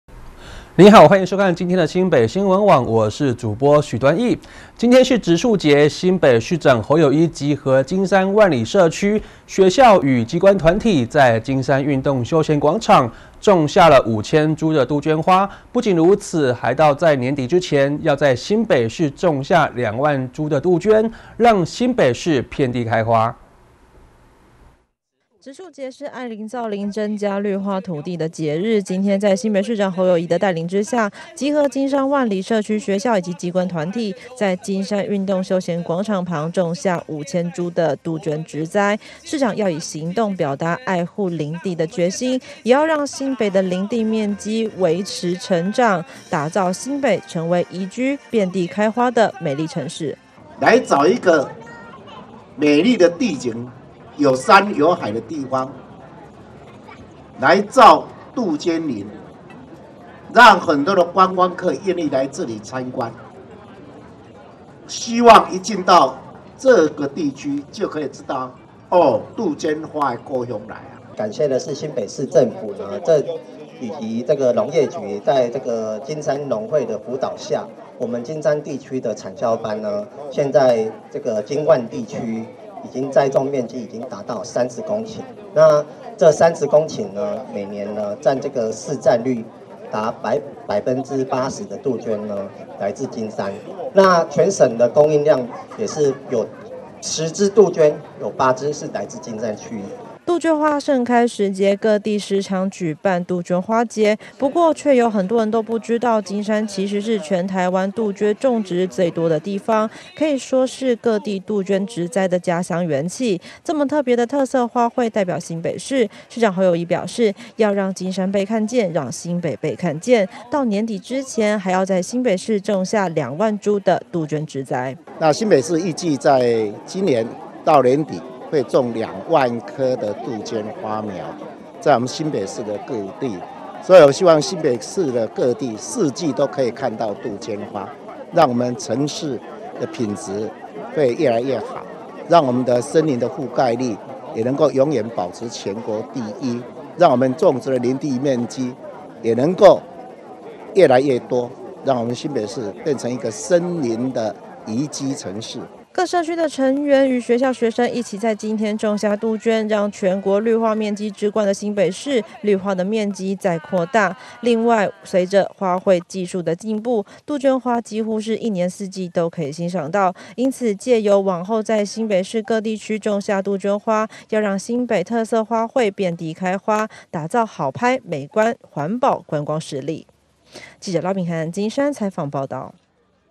植樹節種五千株杜鵑 讓新北遍地開花
今（12）日植樹節，為了要以行動表達愛護林地的決心，也要讓新北的林地面積持續成長，打造新北成為宜居、遍地開花的美麗城市，在新北市長侯友宜的帶領下，集合金山萬里社區、學校與機關團體，於金山運動休閒廣場種下五千株的杜鵑花，不僅如此到年底之前，還要在新北市種下兩萬株的杜鵑，讓新北遍地開花。

杜鵑花盛開時節，各地時常辦理杜鵑花節，不過卻有很多人不知道，金山其實是全台灣杜鵑種植最多的地方，可以說是各地杜鵑植栽的家鄉起源，這麼特別的特色花卉代表新北市，市長侯友宜表示，要讓金山被看見、讓新北被看見，到年底之前要在新北市種下兩萬株的杜鵑。

新北市長侯友宜表示，植樹節是愛林造林增加綠化土地的節日，今天號招金山萬里社區、學校與機關團體，準備在金山運動休閒廣場旁種下五千株的杜鵑植栽，要以行動表達愛護林地的決心，也要讓新北的林地面積持續成長，打造新北成為宜居、遍地開花的美麗城市。

各社區成員與學校學生一起在今天種下杜鵑，讓全國綠化面積之冠的新北市，綠化面積再擴大，另外隨著花卉技術的進步，杜鵑花幾乎一年四季都可以欣賞到，因此藉由往後在新北市各地區種下杜鵑花，讓新北特色花卉遍地開花，打造好拍、美觀的環保觀光實力。